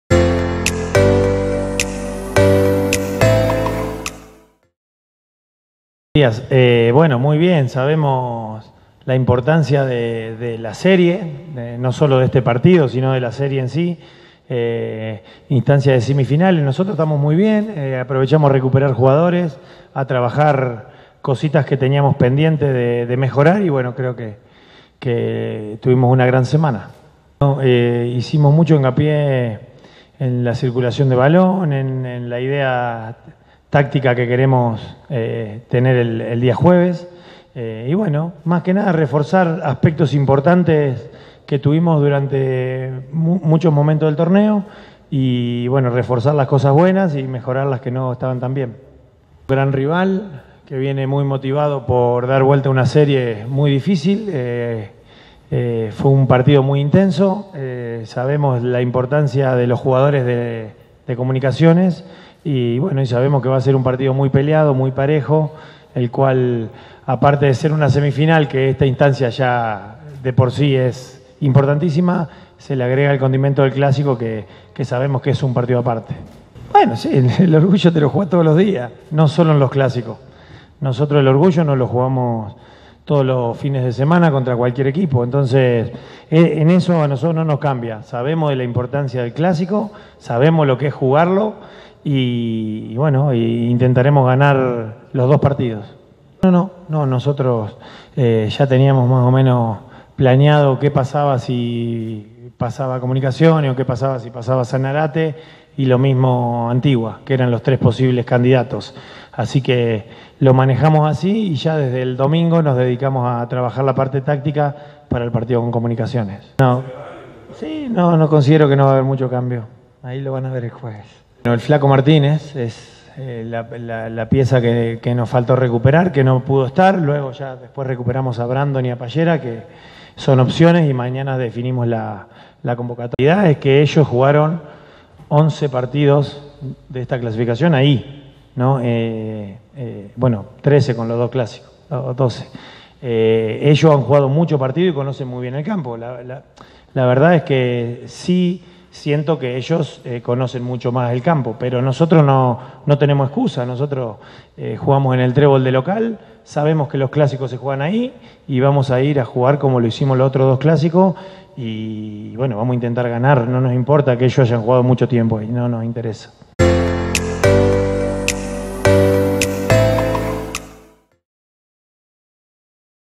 Sebastián Bini
El técnico rojo analizó el juego ante los Cremas.